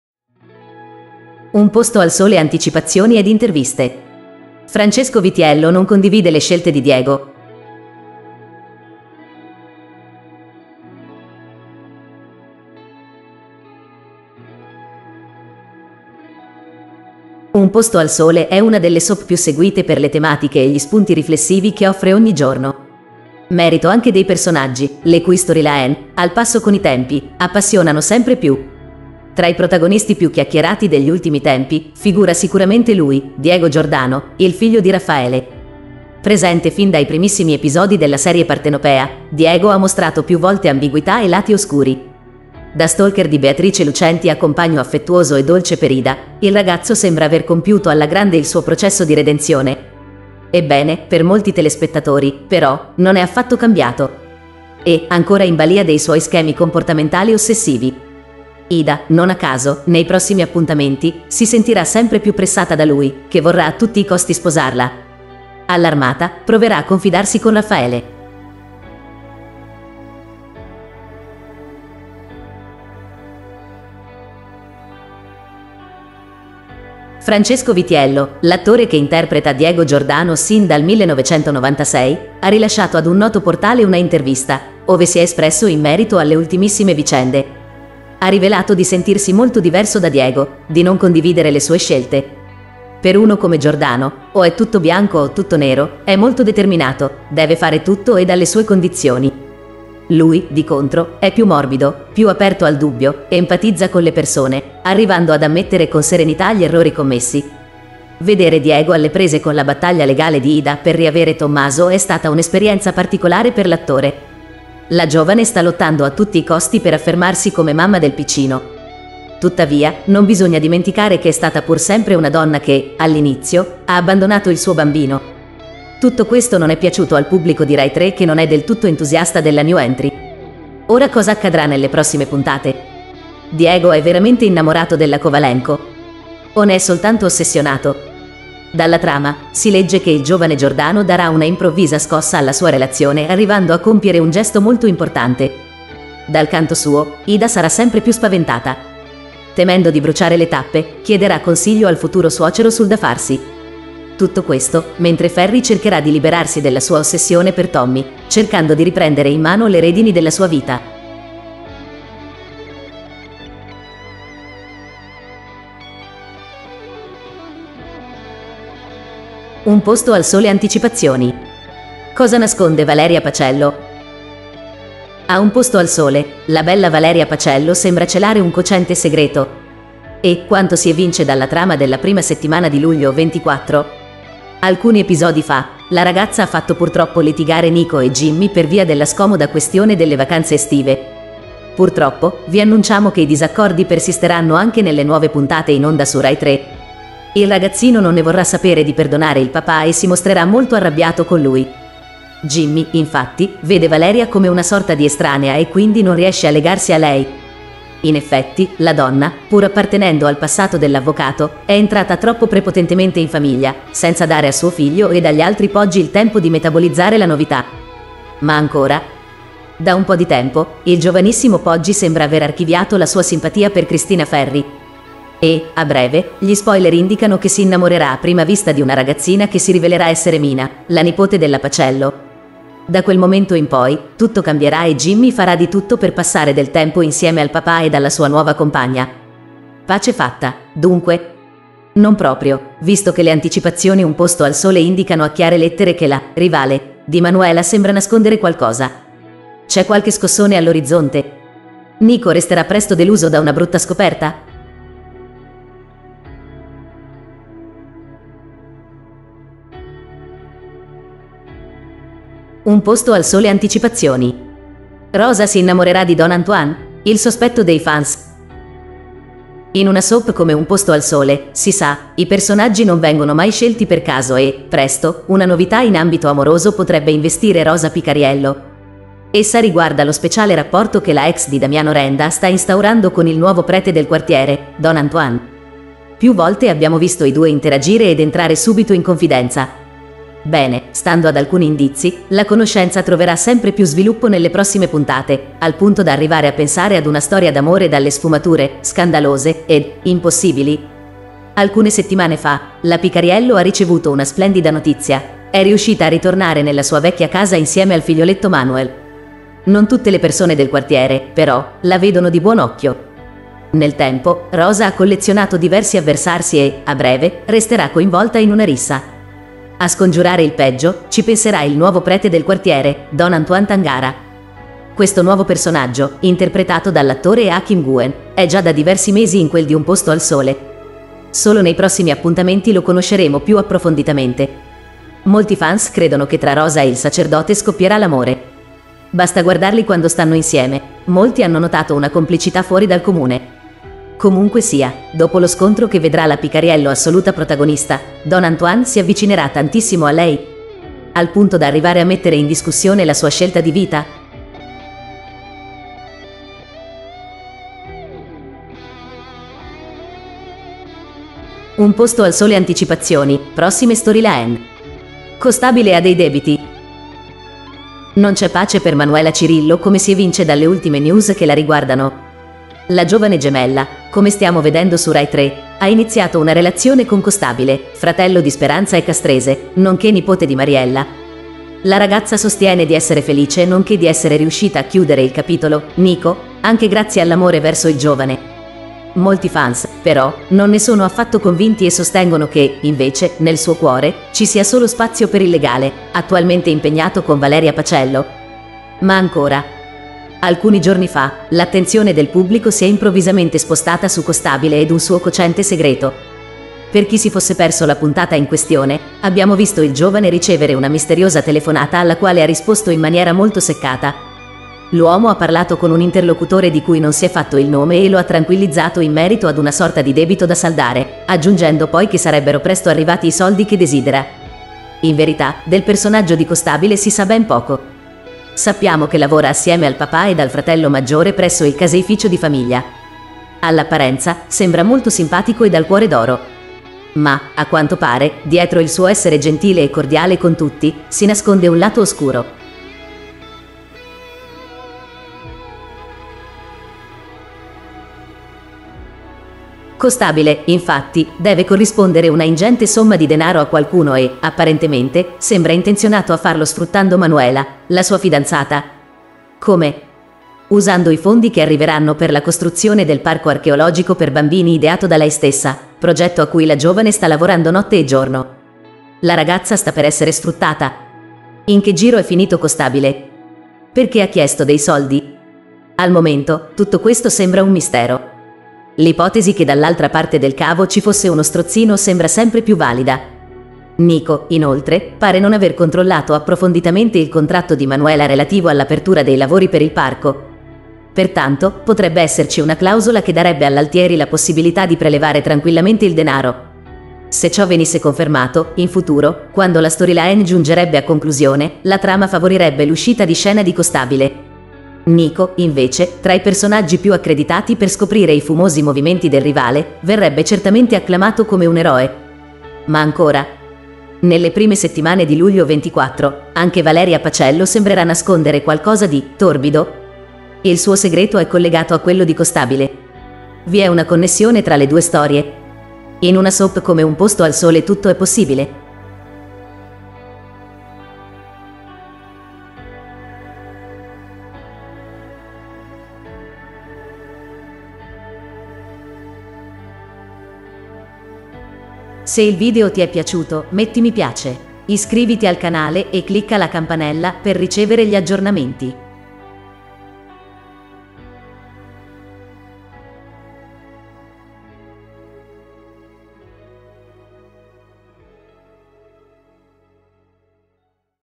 Un Posto al Sole: Francesco Vitiello non condivide le scelte di Diego!
Un Posto al Sole: Un posto al sole è una delle soap più seguite per le tematiche e gli spunti riflessivi che offre ogni giorno. Merito anche dei personaggi, le cui storyline, al passo con i tempi, appassionano sempre più. Tra i protagonisti più chiacchierati degli ultimi tempi, figura sicuramente lui, Diego Giordano, il figlio di Raffaele.